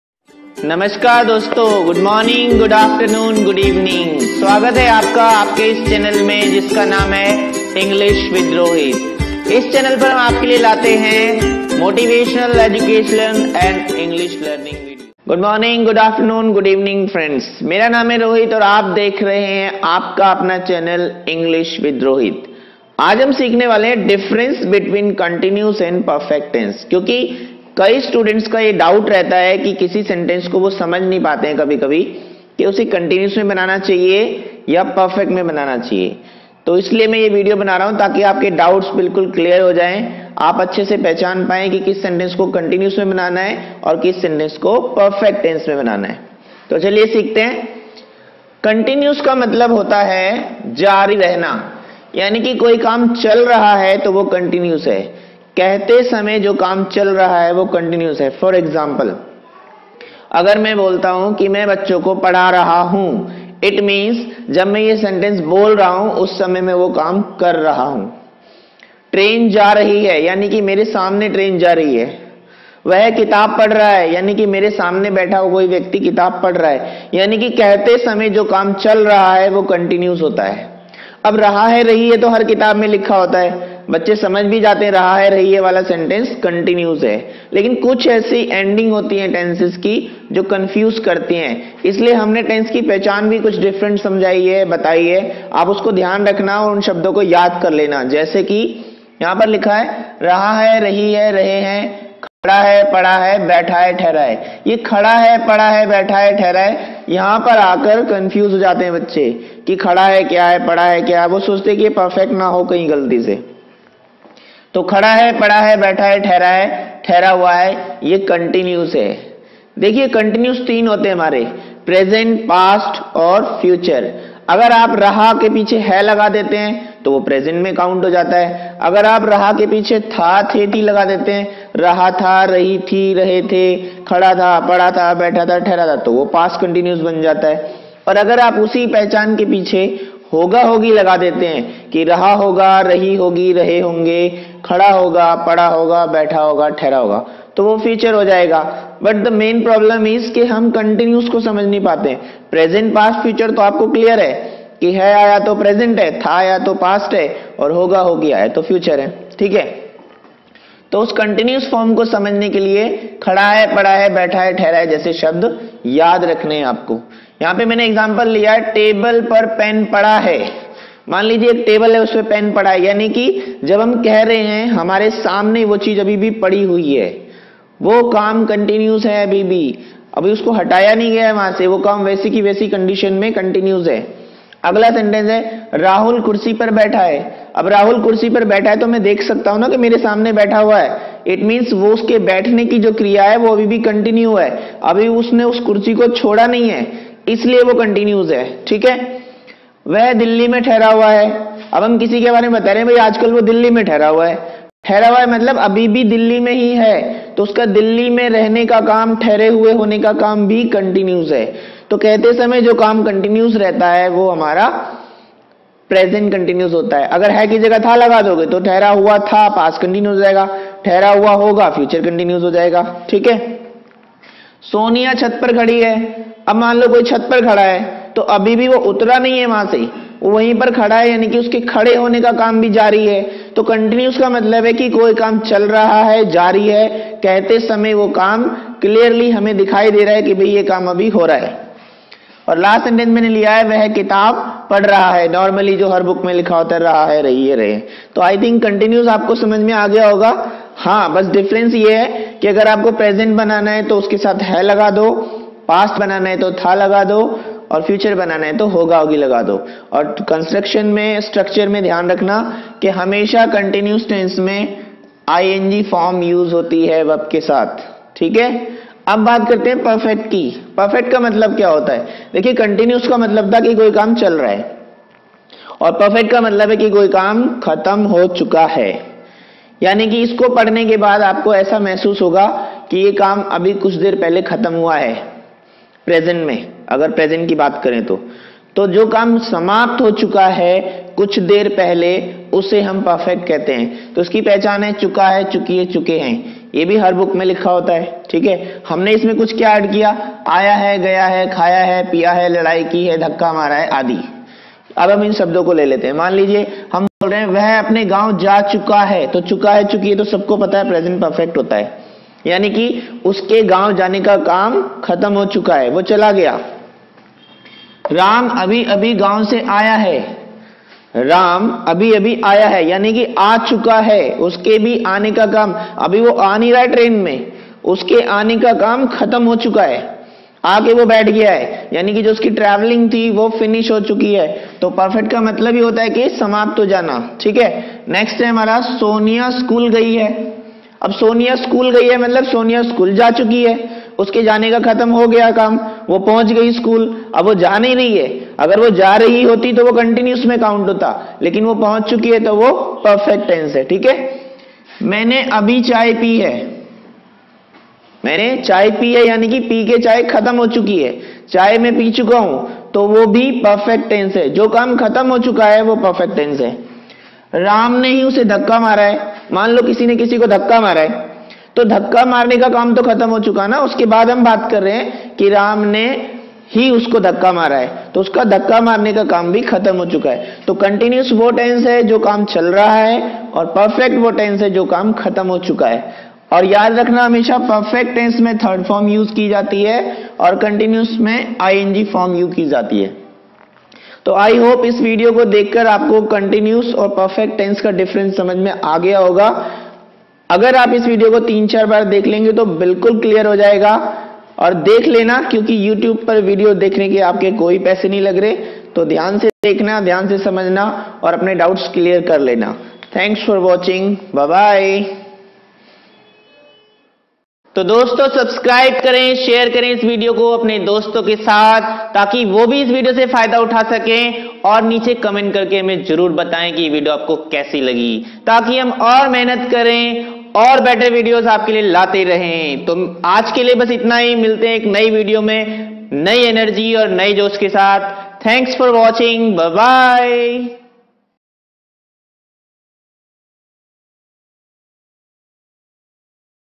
Tense English grammar learning|perfect tense|continuous tense
In this video you are going to learn  difference between continuous and perfect tense. What is continuous tense. What is perfect tense. You will be able to learn the difference between continuous and perfect tense.